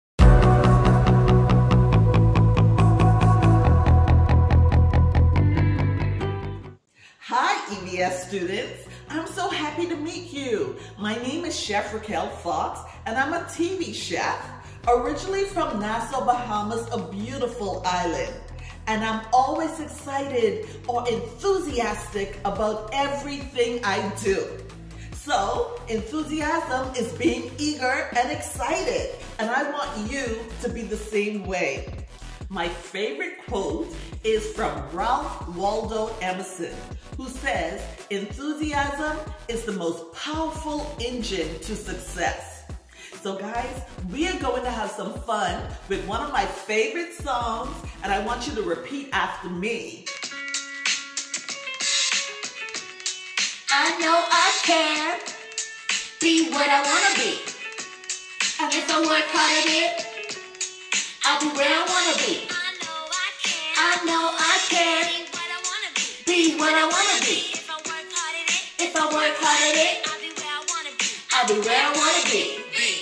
Friday Feature March 10
This week we are featuring Raquel Fox, a chef, who is featured on Cityline and Breakfast Television.   Her message to our students is "Enthusiasm is one of the most powerful engines of success" by Ralph Waldo Emerson.